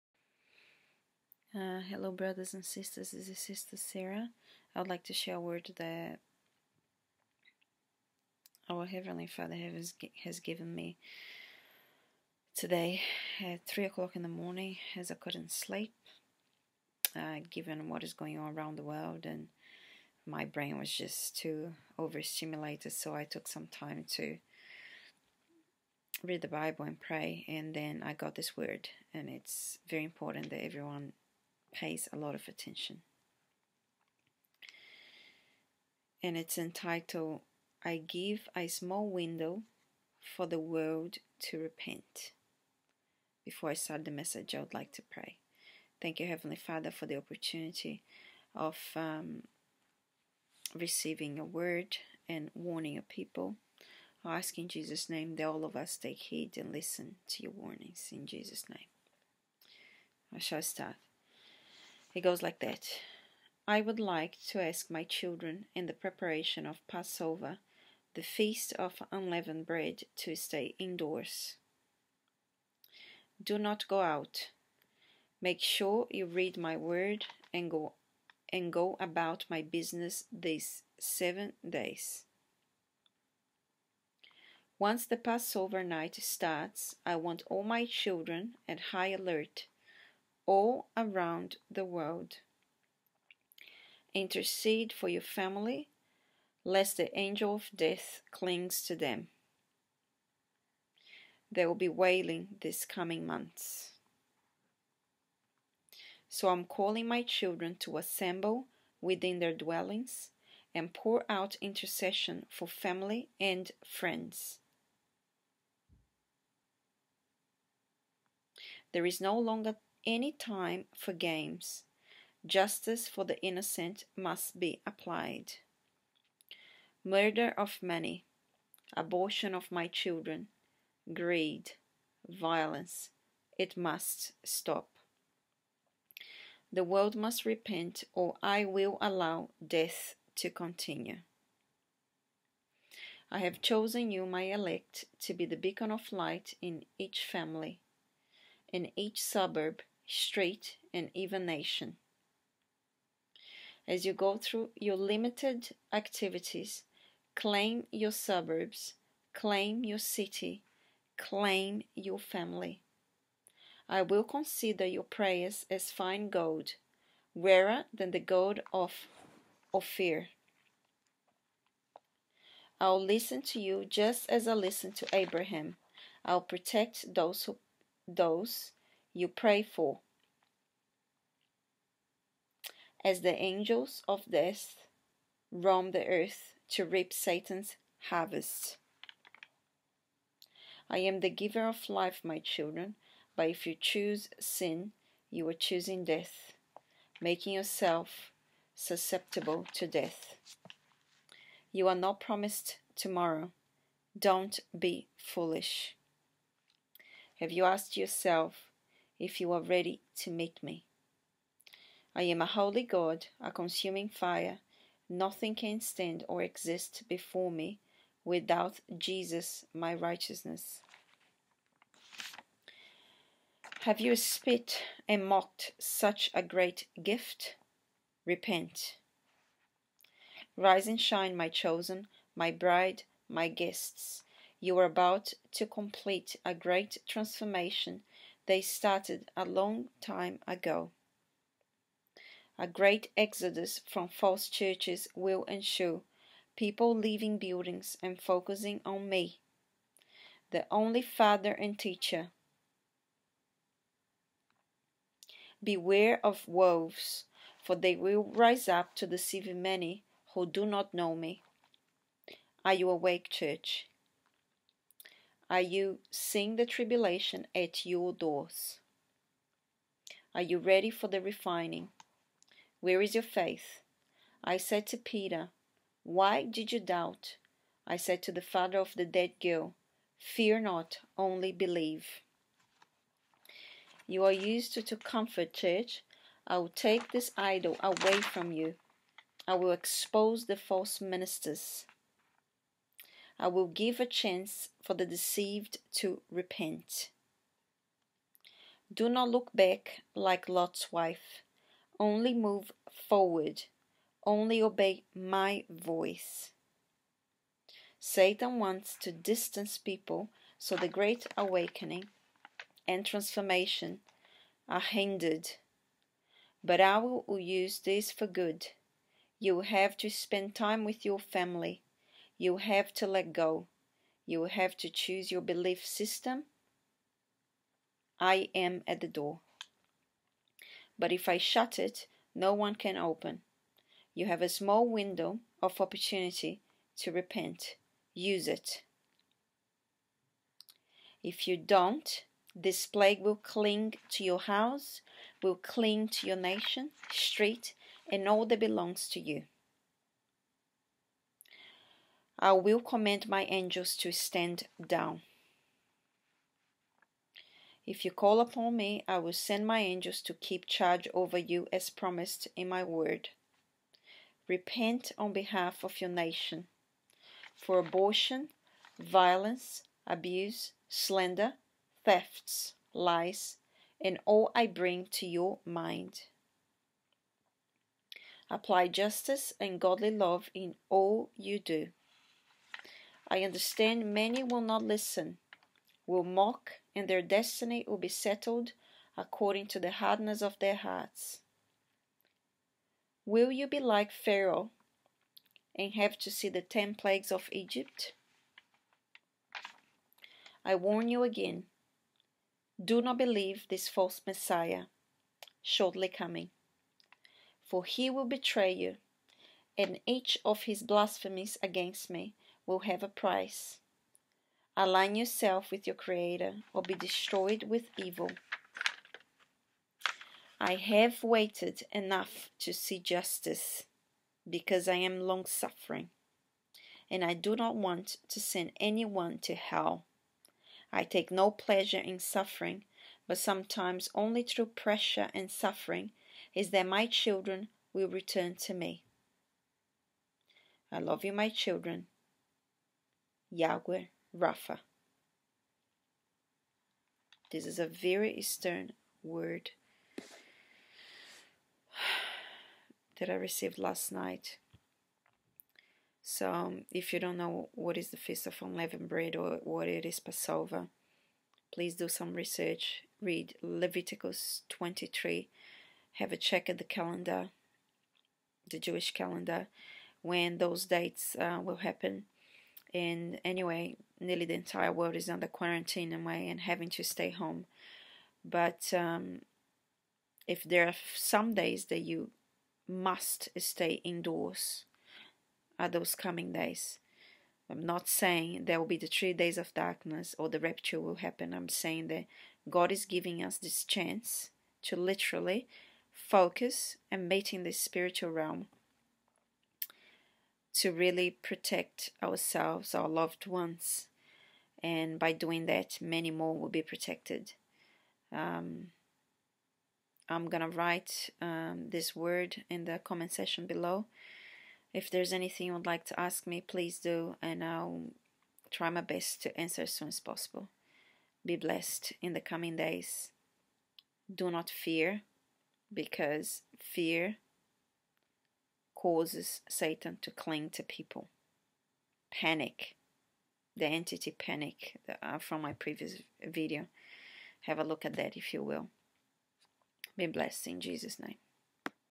WORD:STAY INDOORS THIS PASSOVER FEAST 2020- A SMALL WINDOW TO REPENT
Word from our Heavenly Father

24/03/2020

"I give a small window for the world to repent"

I would like to ask my children in the preparation for passover. In the feast of unleavened bread- to stay indoors.Do not go out. Make sure you read my word and go about my business these 7 days. Once the passover night starts I want all my children at high alert all around the world.

Intercede for your family, lest the angel of death clings to them. There will be wailing these coming months. So, I'm calling my children to assemble within their dwellings and pour out intercession for family and friends. There is no longer any time for games, justice for the innocent must be applied: The murder of many, abortion of my children, greed, violence. It must stop. The world must repent or I will allow death to continue

I have chosen you my elect to be the beacon of light in each family. In each suburb/street and even nation. As you go through your limited activities: claim your suburb/claim your city/claim your family. I will consider your prayers as fine gold- rarer than the gold of ophir.

I will listen to you just as I listened to Abraham. I will protect those you pray for as the angels of death roam the earth to reap satan's harvest. I am the giver of life, but if you choose sin, you are choosing death. Making yourself susceptible to death.

You are not promised tomorrow. Don't be foolish. Have you asked yourself if you are ready to meet me? I am a Holy God, a consuming fire. Nothing can stand or exist before me without Jesus my righteousness. Have you spit and mocked such a great gift? REPENT!

Rise and shine my chosen, my bride, my guests. You are about to complete a great transformation that started a long time ago. A great exodus from false churches will ensue, people leaving buildings and focusing on me- the only FATHER and teacher. BEWARE of wolves, they will rise up to deceive many who do not know me. Are you awake church? Are you seeing the tribulation at your doors? Are you ready for the refining? where is your faith??

I said to Peter: Why did you doubt? I said to the Father of the dead girl-- FEAR not, only believe! You are used to comfort church. I will take this idol away from you. I will expose the false ministers. I will give a chance for the deceived to repent.

Do not look back like LOT's wife. Only move forward. Only obey my voice. Satan wants to distance people, so the great awakening and transformation are hindered. But I will use this for good. You will have to spend time with your family. You will have to let go. You will have to choose your belief system. I am at the door. But, if I shut it no one can open-- you have a small window of opportunity to repent

If you don't this plague will cling to your house, will cling to your nation, street and all that belongs to you. I will command my angels to stand down. If you call upon me I will send my angels to keep charge over you as promised in my word.Repent on behalf of your nation for abortion, violence, abuse, slander, thefts, lies and all I bring to your mind. Apply justice and godly love in all you do.

I understand many will not listen, will mock and their destiny will be settled according to the hardness of their hearts. Will you be like pharaoh and have to see the 10 plagues of Egypt?

I warn you again-- do not believe this false messiah coming. For he will betray you, and each of his blasphemies against me will have a price. Align yourself with your creator or be destroyed with evil. I have waited enough to see justice because I'm long suffering, and do not want to send anyone to hell. I take no pleasure in suffering, but sometimes only through pressure and suffering us that my children will return to me.

I love you my children-- YHWH Rapha

what is SIN: read exodus 20
what is passover? Read leviticus 23
2 peter 3:9

The Lord is not slow in keeping his promise, as some understand slowness. Instead he is patient with you, not wanting anyone to perish, but everyone to come to repentance.